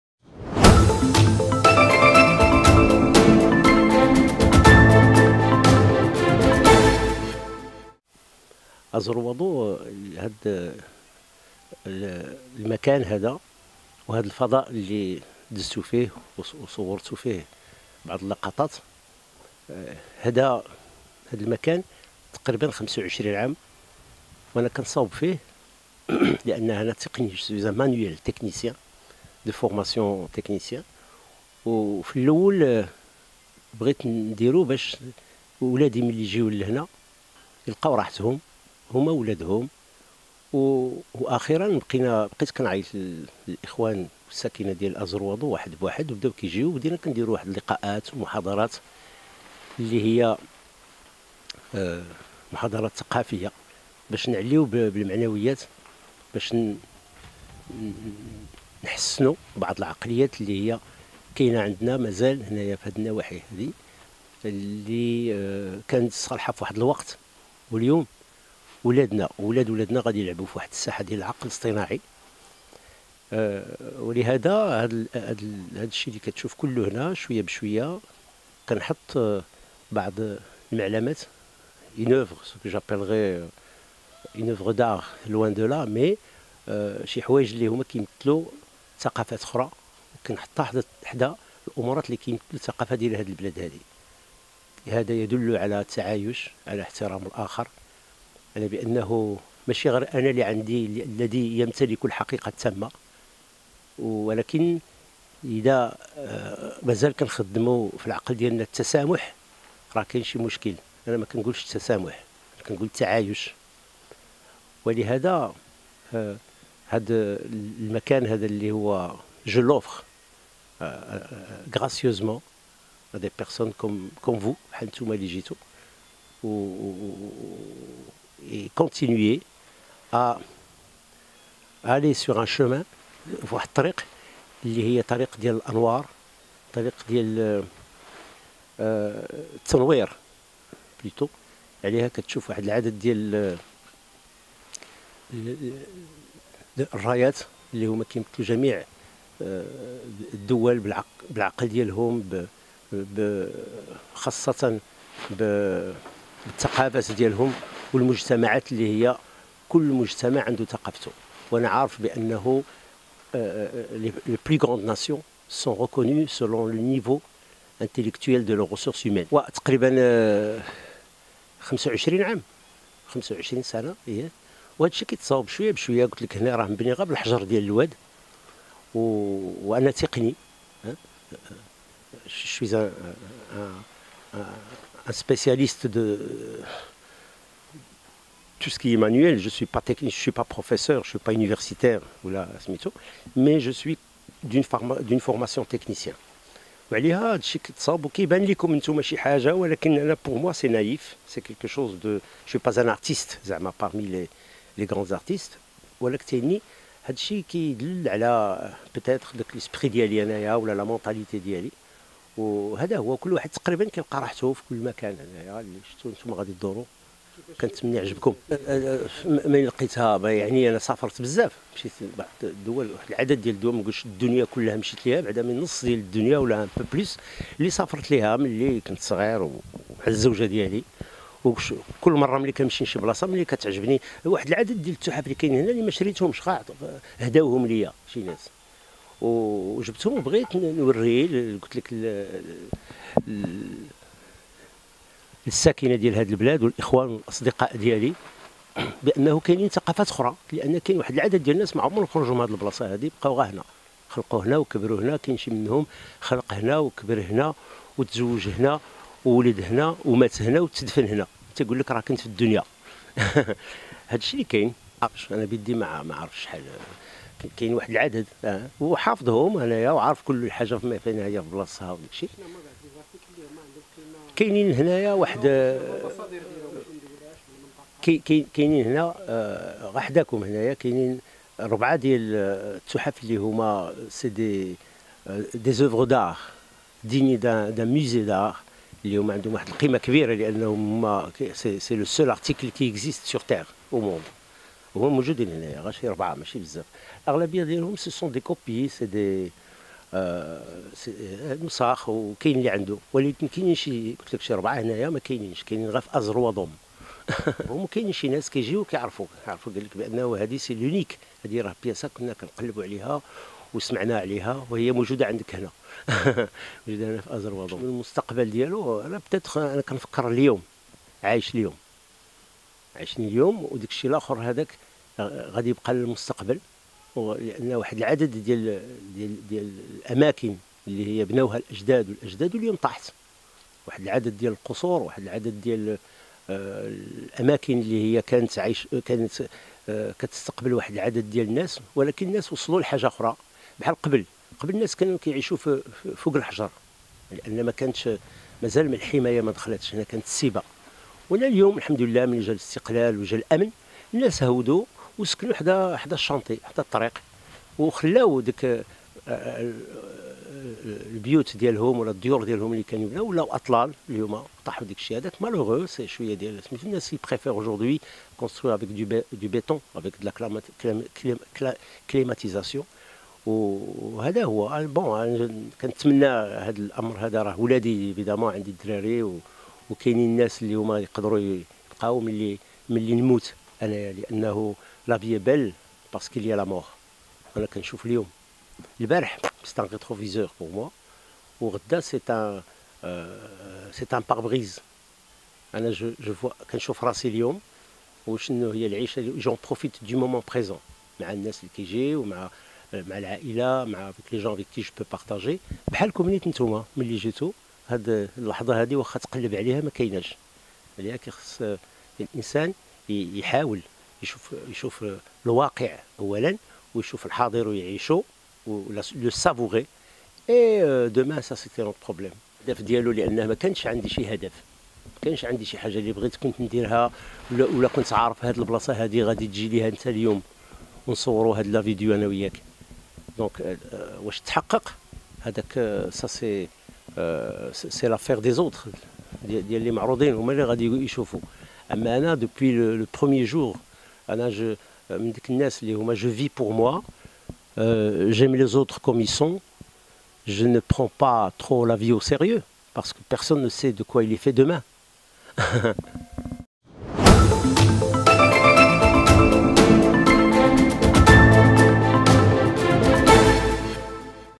منزل يَتَحَوَّلُ إلى مُتْحَفٍ في أرياف تافراوت تطلَّب إنجازُه 25 عاماً
# أخبار_عاجلة